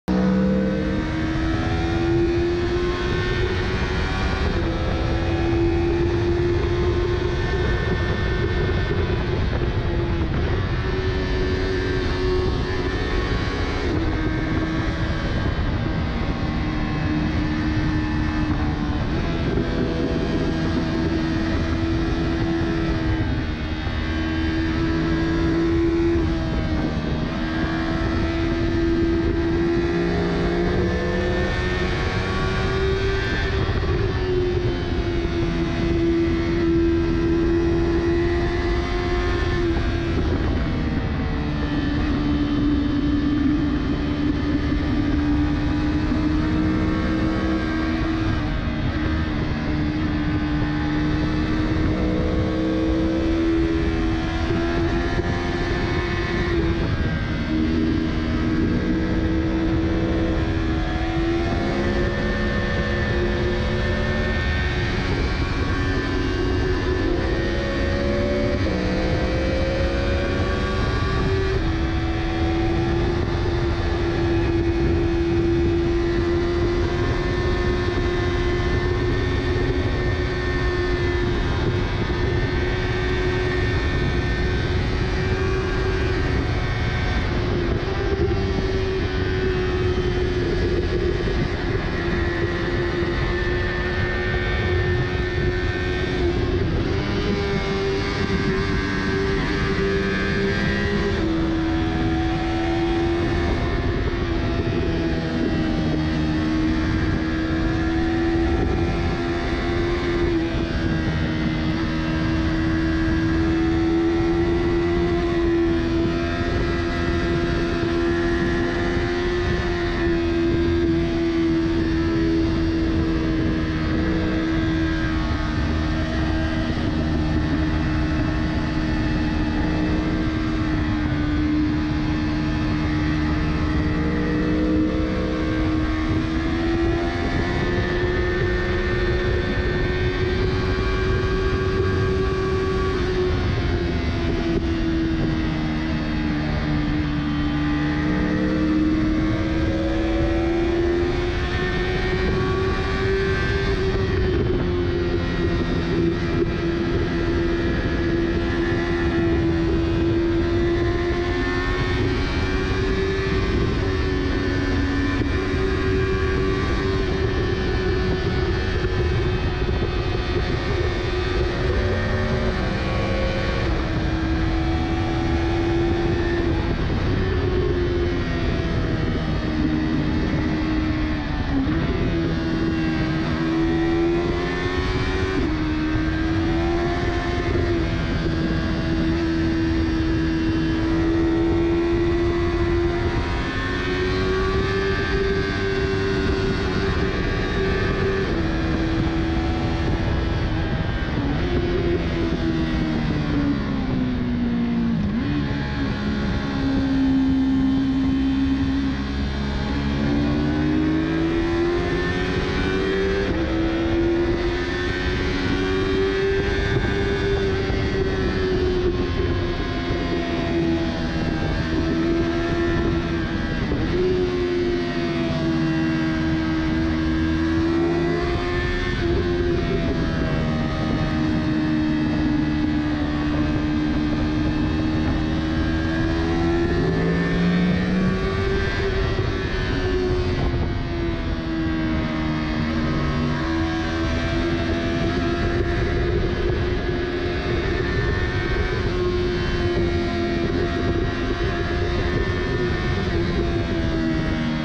Super fast 2019 Yamaha R3 chasing an Aprilia Tuono 1100 V4 Factory on Angeles Crest
MrPacisnod chasing me on his super quick Yamaha R3. Tells you that you that it's the rider that makes the difference, not the bike!